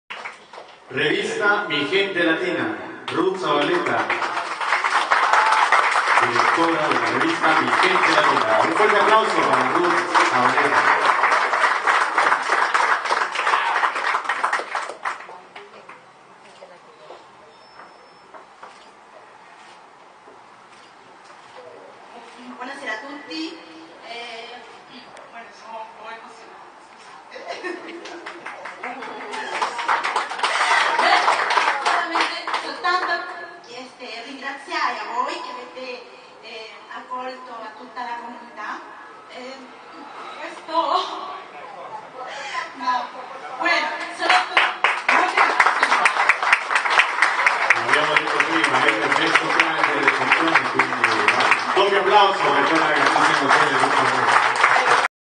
INTEGRAZIONE E IMMIGRAZIONE DELLA COMUNITA' PERUVIANA IN ITALIA ruth zavaleta.MPG
"INTEGRAZIONE E IMMIGRAZIONE DELLA COMUNITA' PERUVIANA IN ITALIA" Venerdì 17 Giugno 2011, Ore. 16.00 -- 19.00 Sala Consiliare del Municipio XVII - Roma,